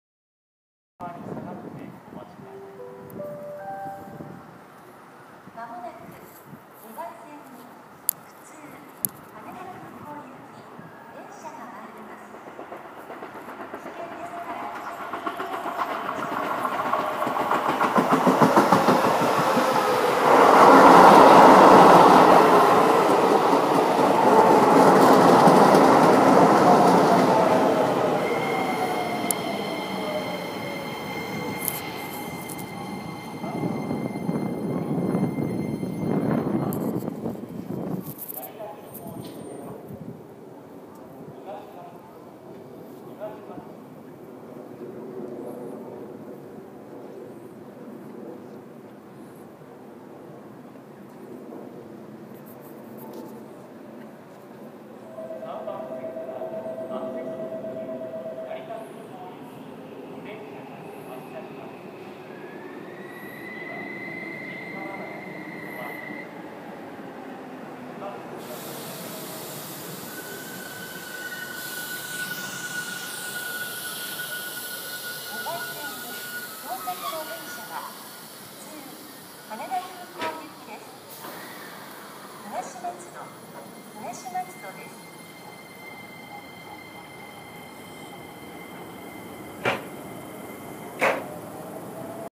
東松戸駅に到着する下りアクセス特急に上り普通
北総線・成田スカイアクセス線（京成成田空港線）東松戸駅に到着する下りのアクセス特急成田空港行きに上り普通羽田空港行き。アクセス特急は京成3050形、普通は北総9000形でした。